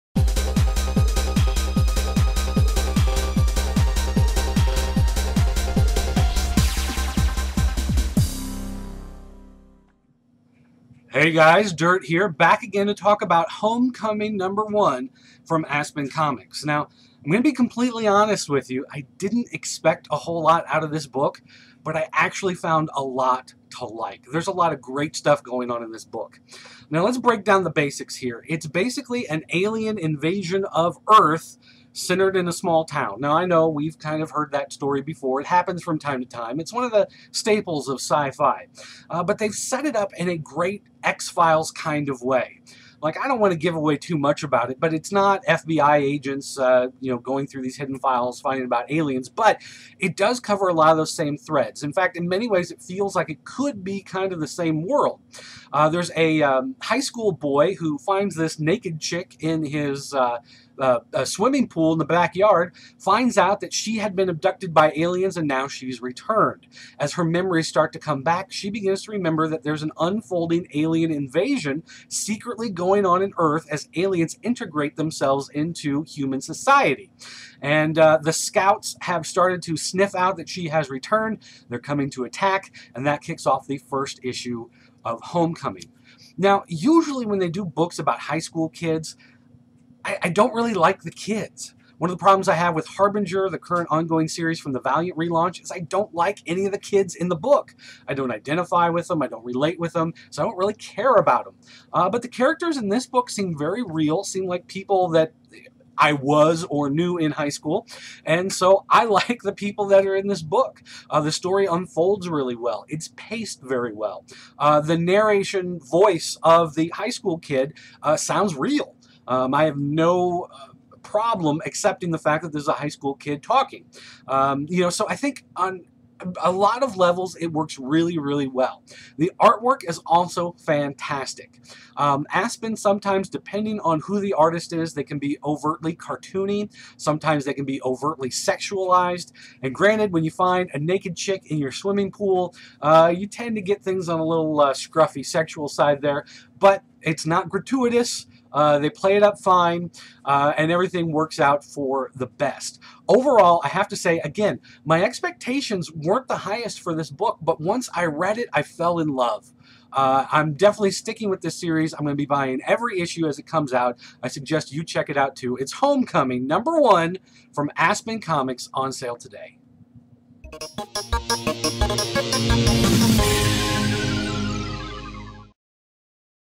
Homecoming #1 Review
DiRT takes a look at the latest series from Aspen Comics - an alien abduction thriller that wonderfully fulfills a lot of X-Files fantasies! Sure, it's a got a naked woman and alien killers, but it also features a group of high schoolers you actually kinda like!